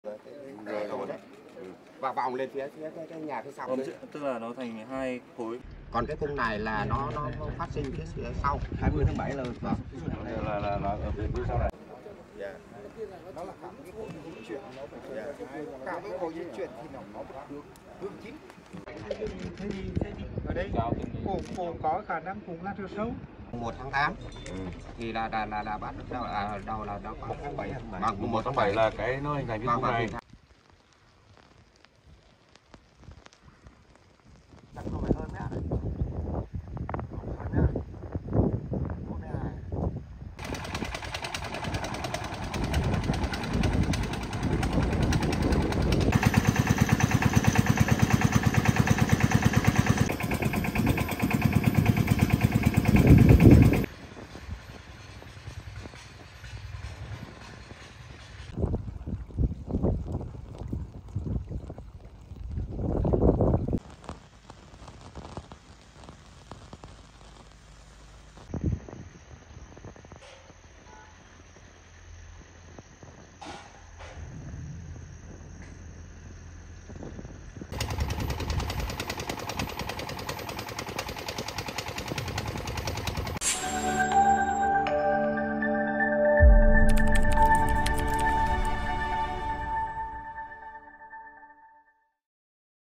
Thứ trưởng Bộ NN-PTNT kiểm tra sạt lở tại hồ Đông Thanh (Lâm Đồng)
Ngày 8-8, Thứ trưởng Bộ NN-PTNT Nguyễn Hoàng Hiệp và đoàn công tác Ban Chỉ đạo quốc gia về phòng, chống thiên tai, cùng lãnh đạo tỉnh Lâm Đồng, các chuyên gia địa chất kiểm tra tình trạng nứt, sụt đất tại dự án hồ chứa nước Đông Thanh, huyện Lâm Hà.